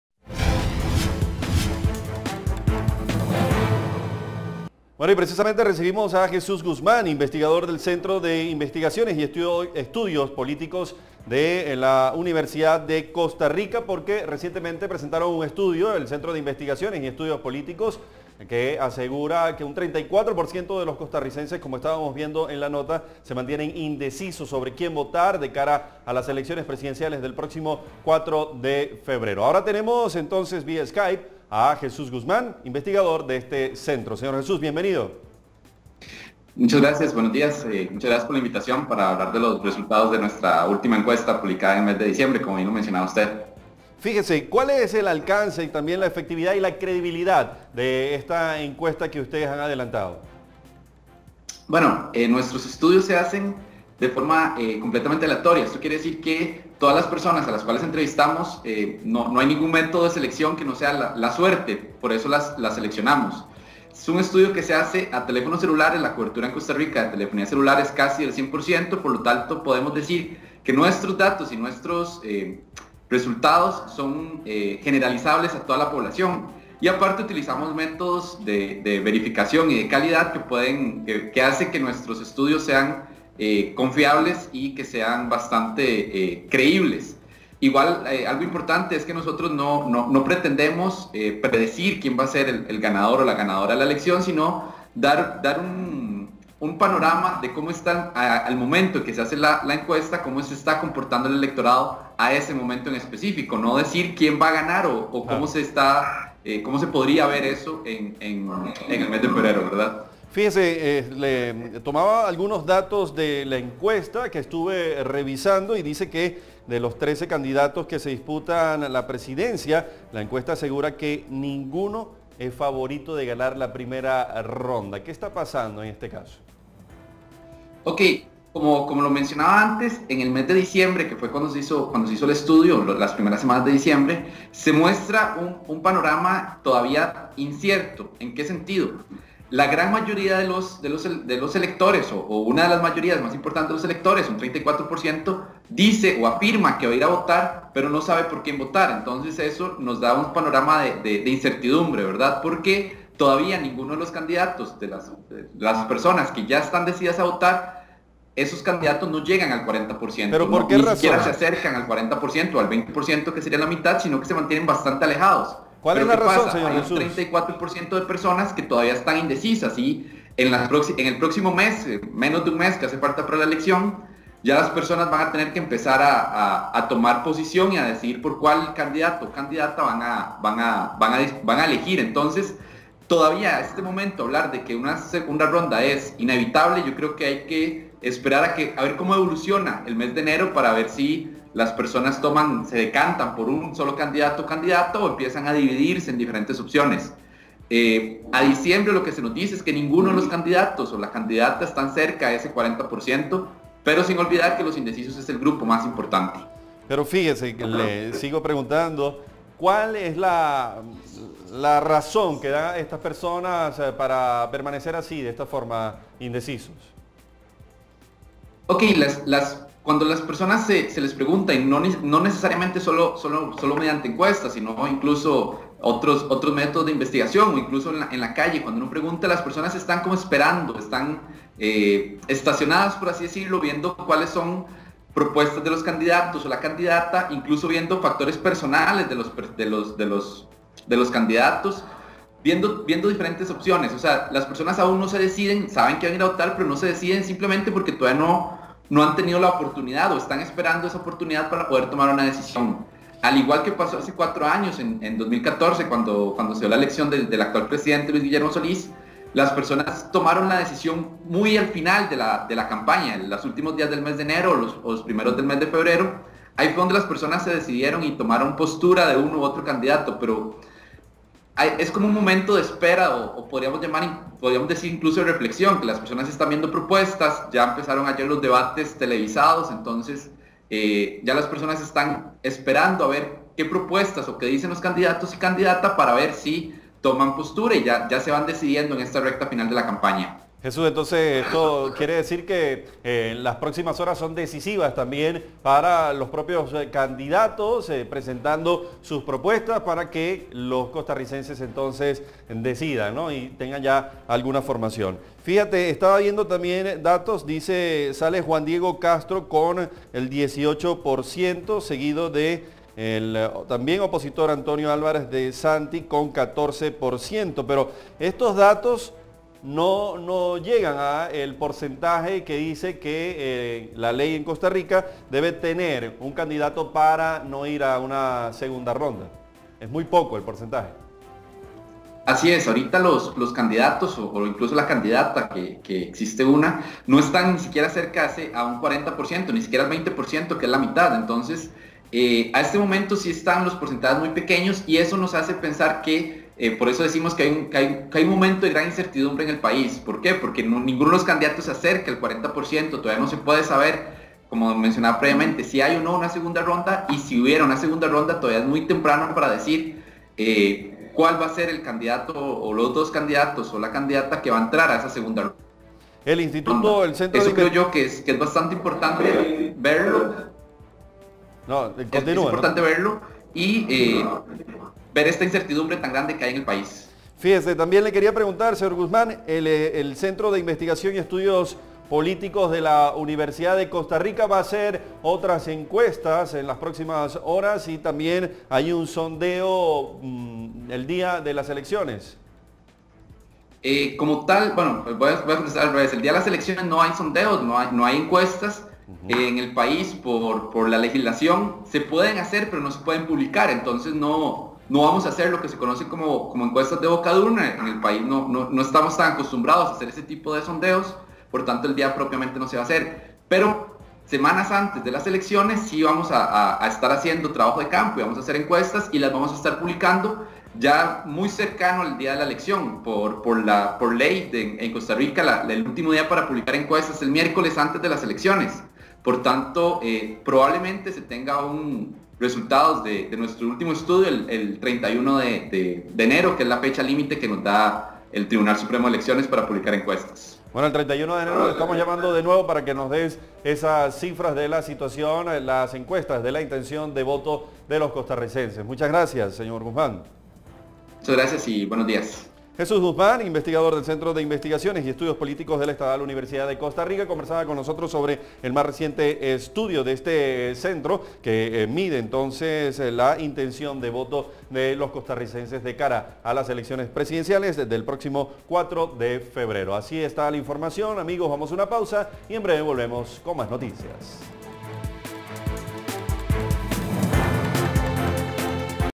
34% de los costarricenses no sabe por quién votar en próximas elecciones según estudio
A menos de un mes de las elecciones generales en Costa Rica, 34% de los ciudadanos empadronados del país centroamericano se mantienen indecisos sobre por quién votar, según un estudio del Centro de Investigación y Estudios Políticos de la estatal Universidad de Costa Rica.

El mismo estudio aseguró que el candidato del Partido Integración Nacional, Juan Diego Castro, lidera la intención de voto con un 18%, sin embargo, este aún se mantiene lejos del 40 por ciento que se requiere para ganar la presidencia de Costa Rica en primera ronda.

Para ampliar en este tema, conversamos con Jesús Guzmán, investigador de este centro.

Redacción / CB24